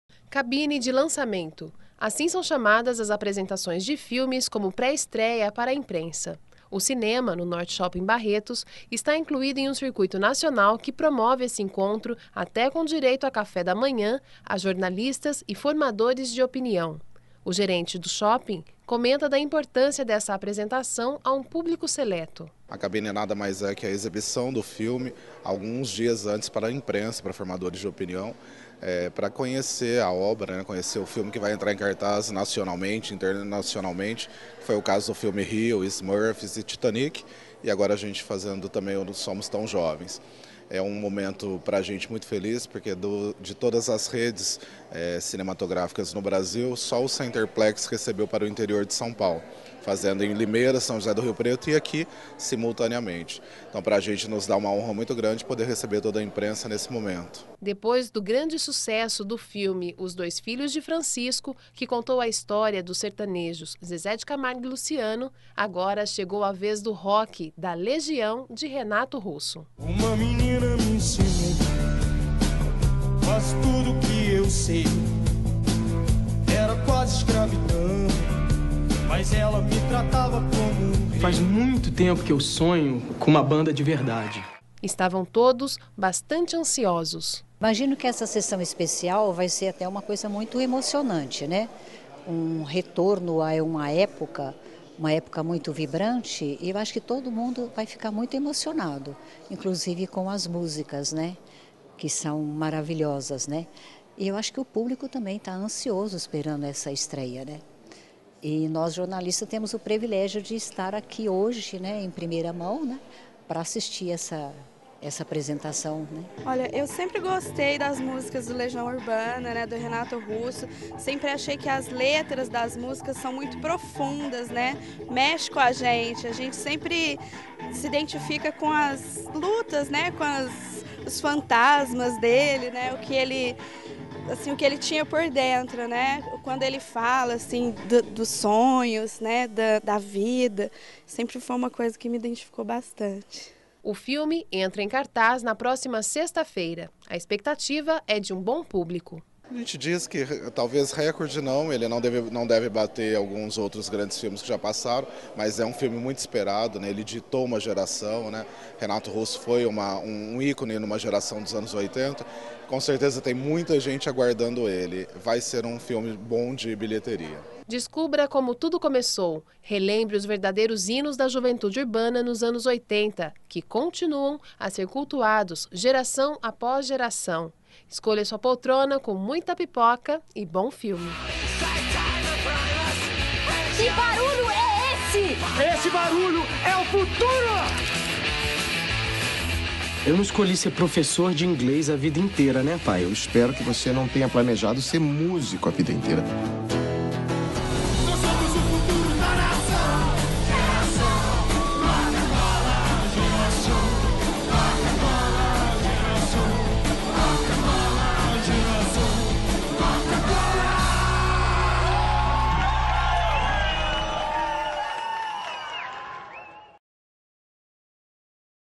Cabine de Imprensa Filme
A imprensa barretense ganhou um presente especial no feriado do Dia do Trabalho: uma sessão de cinema oferecida pelo North Shopping Barretos. Foi exibido o filme "Somos tão jovens" que estréia no circuito nacional nesta sexta-feira (03 de maio de 2013).
Reportagem veiculada nos telejornais da TV Barretos no dia 02 de maio de 2013.